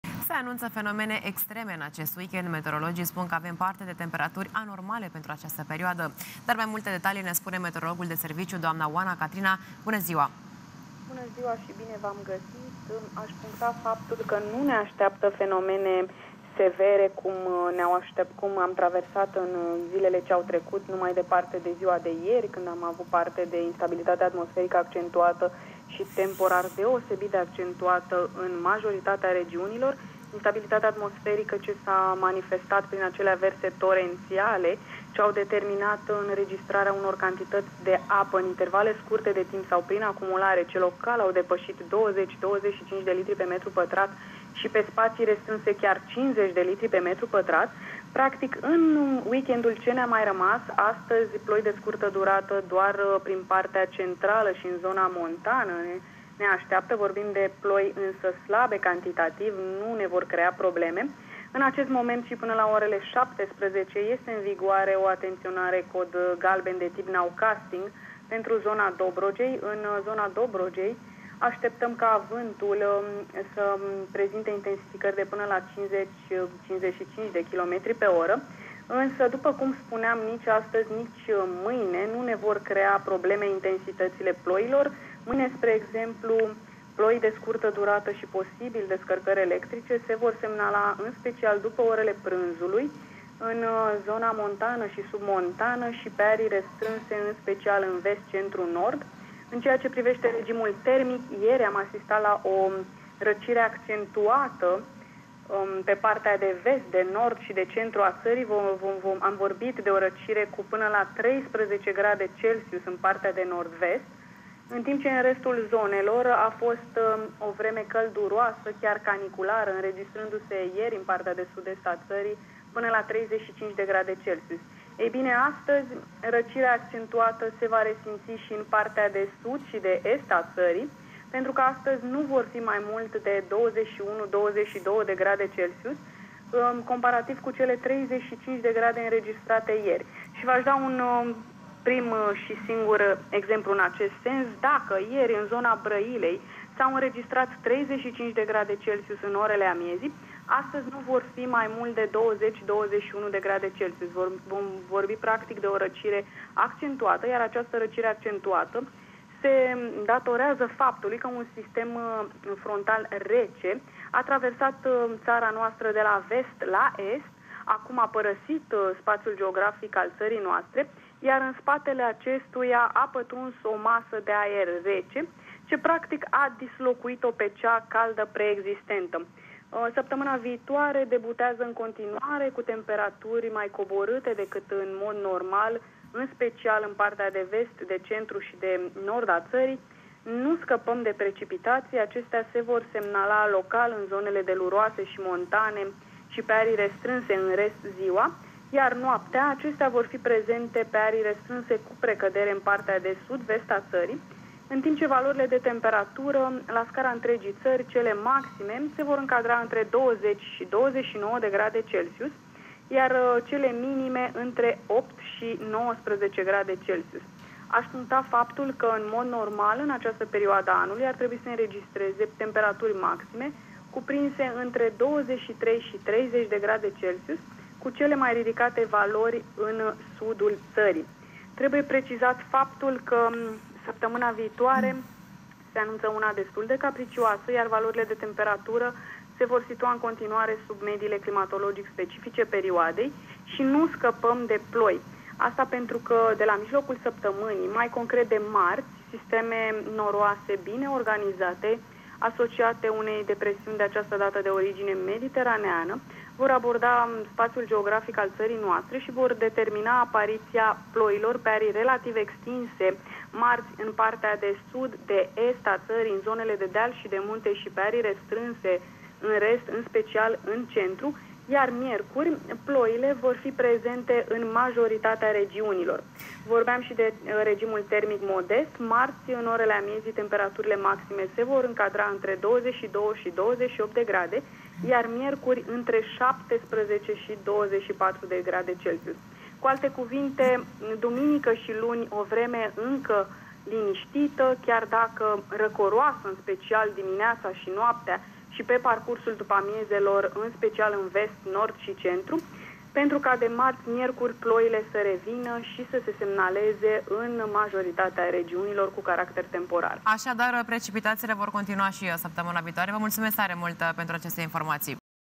Informații de la meteorologi! Temperaturile scad cu 10 grade Celsius
Se anunţă fenomene extreme în acest weekend! Meteorologii spun că avem parte de temperaturi anormale pentru această perioadă.